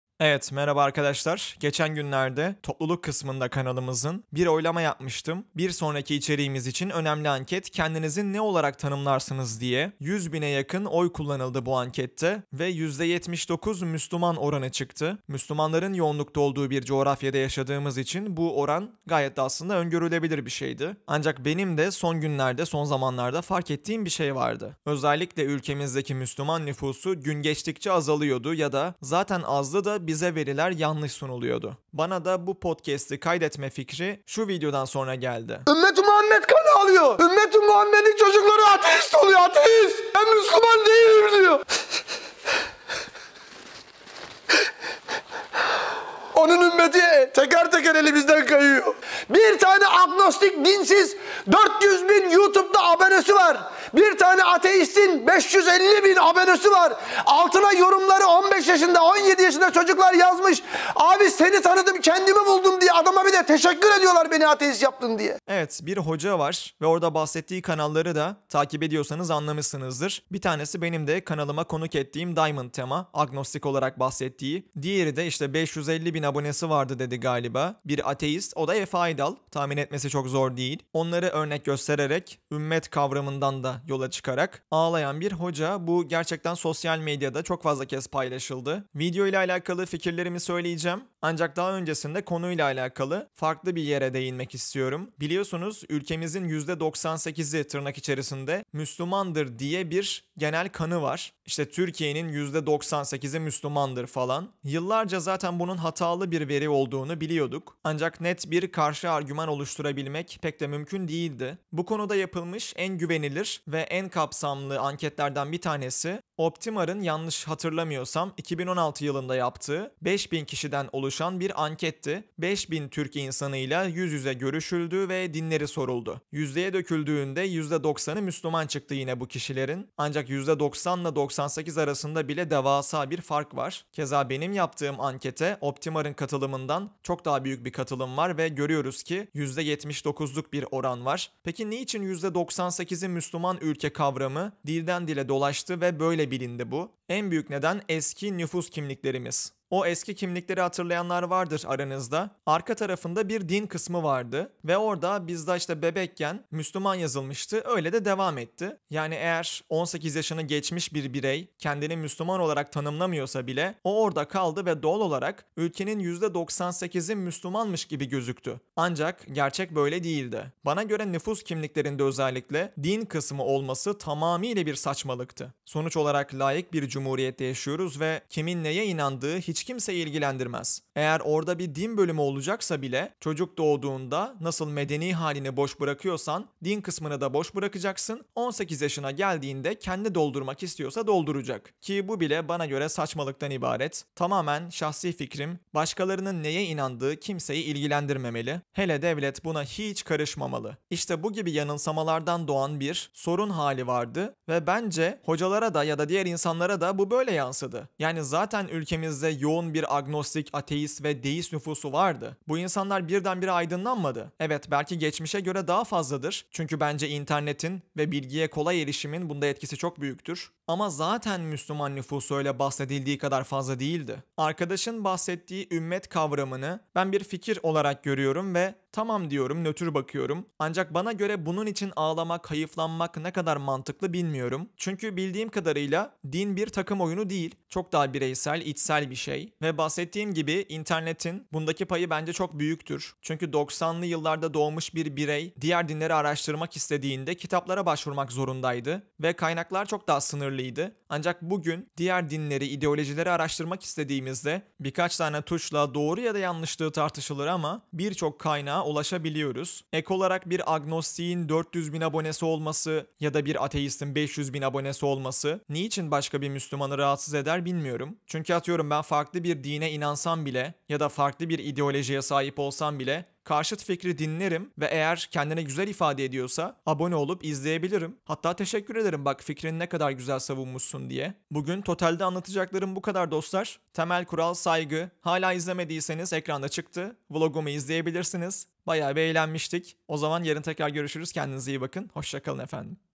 BU HOCA NEDEN AĞLIYOR?
Diamond tema ve efe aydal'ı örneklendirerek ağlayan hoca ve müslüman nüfusunun Türkiye'deki durumu hakkında.

Seslendirir, sunar, kendi çapında yazar.
Enes Aslanlı, Marmara Üniversitesi Gazetecilik öğrencisi. İpe sapa gelmez düşüncelerini mikrofona anlatan 19 yaşındaki Türk genci.